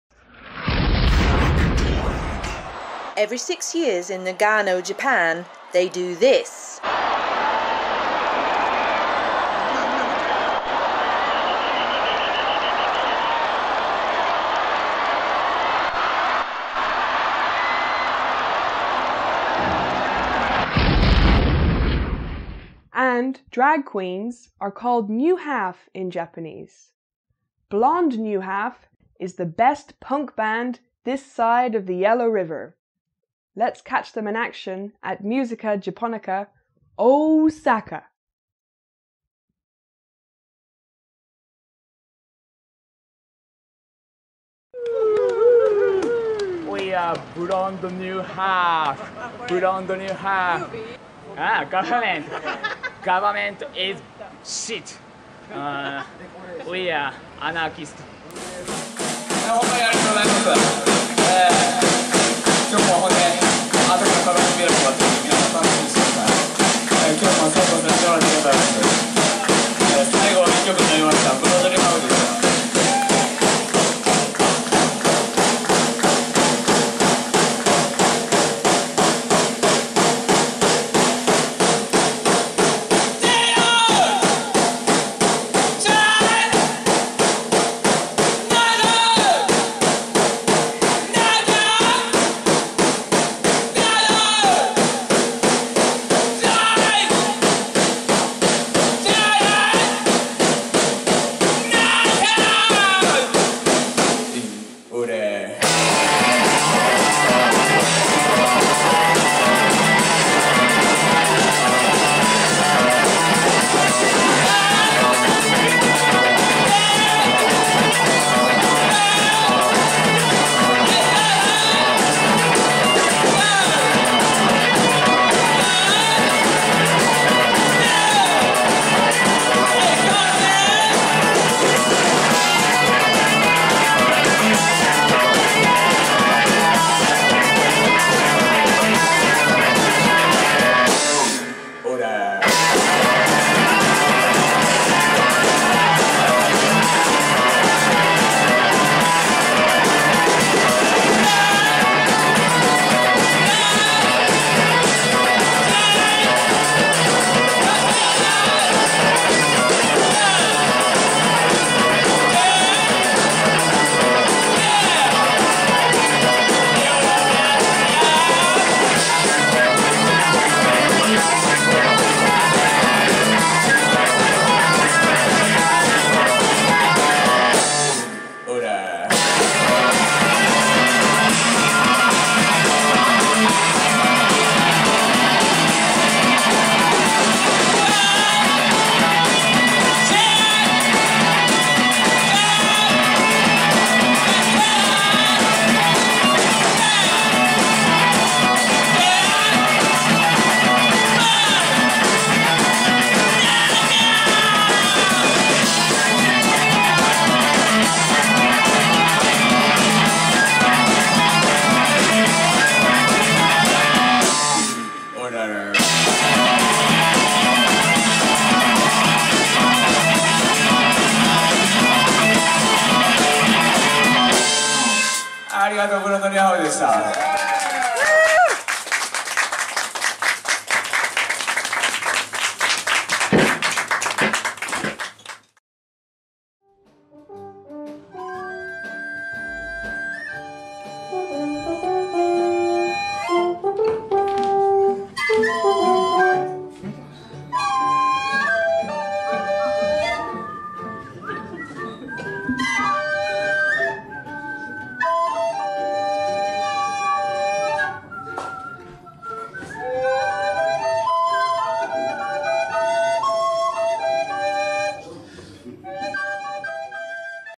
Blond New Half @ Musica Japonica Osaka, Japan
-Edited by Squid Boy-

Blond New Half, the best punk band this side of the Yellow River, rock out at Musica Japonica in OOOOOsaka Japan !